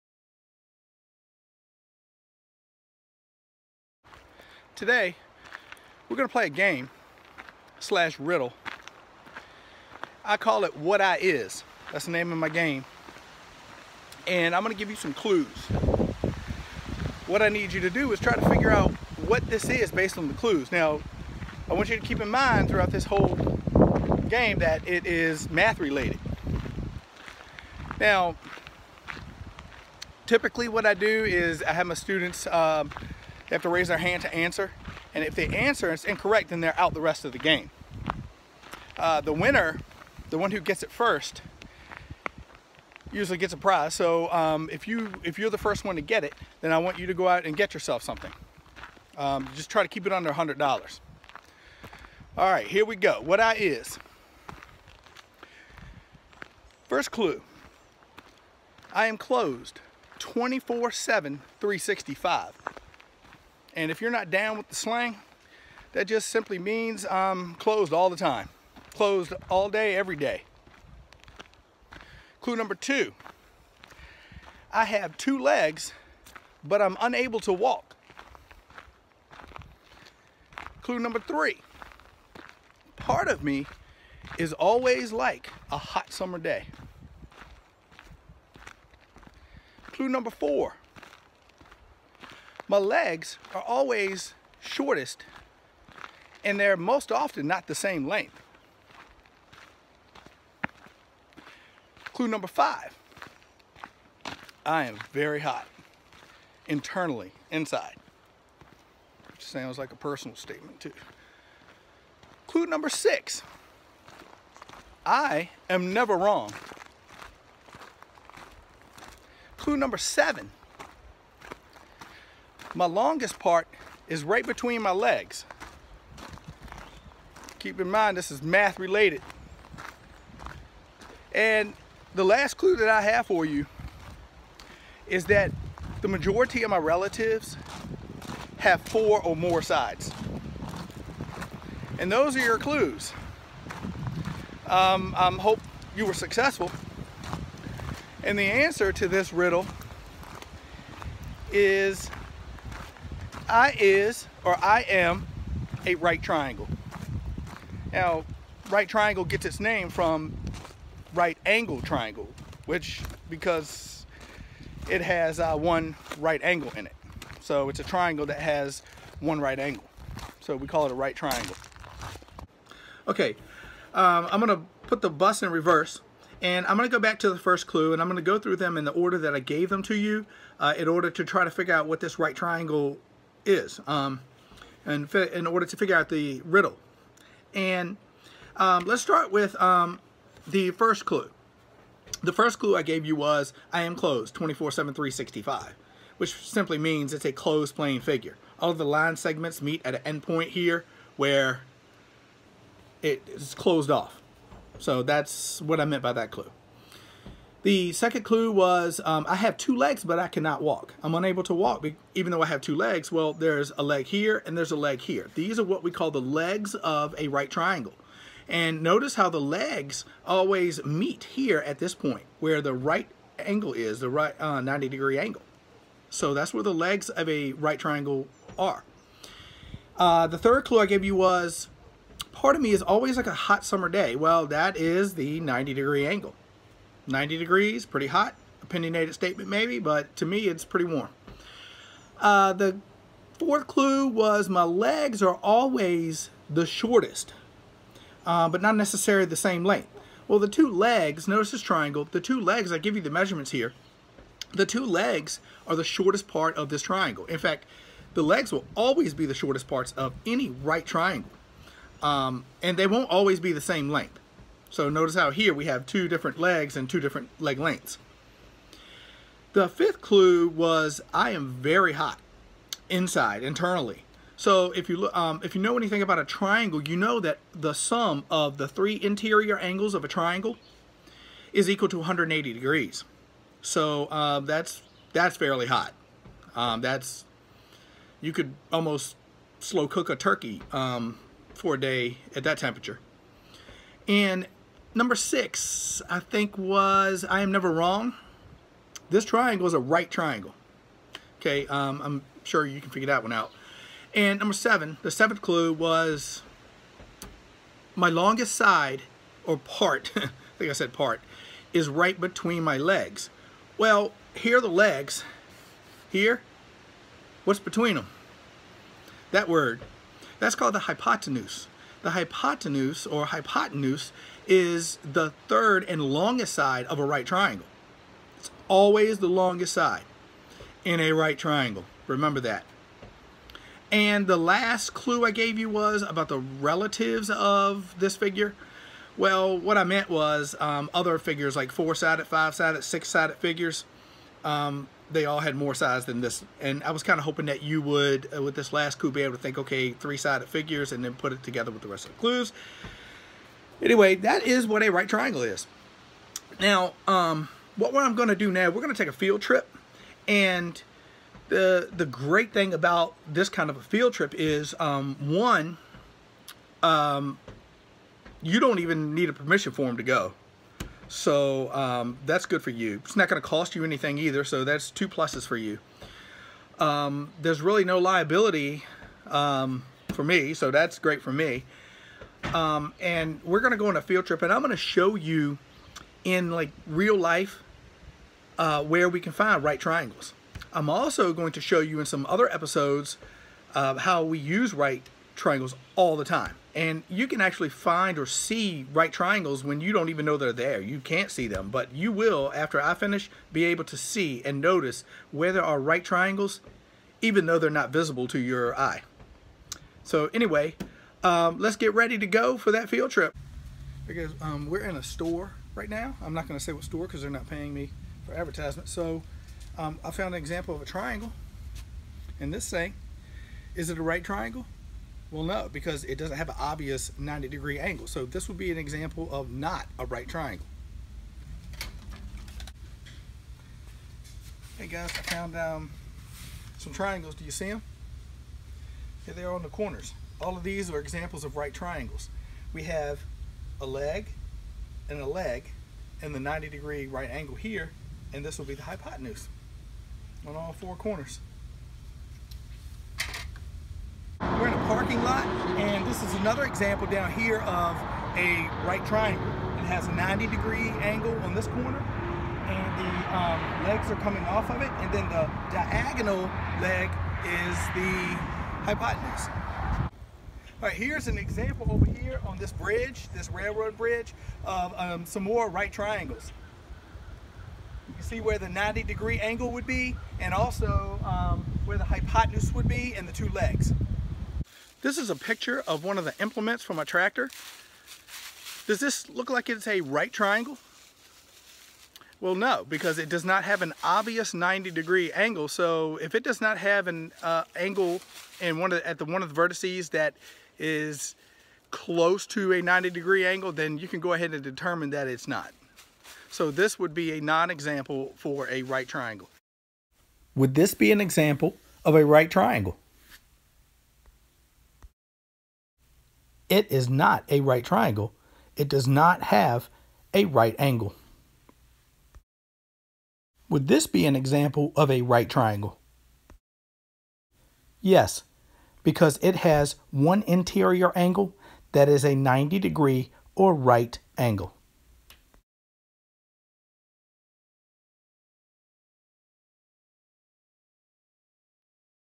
I'm Never Wrong
A game/riddle to introduce the properties (characteristics) of a right angled triangle.  Right triangles have one right angle.  Its sides are called legs (has two) and one side called a hypotenuse.  The hypotenuse is always the longest side.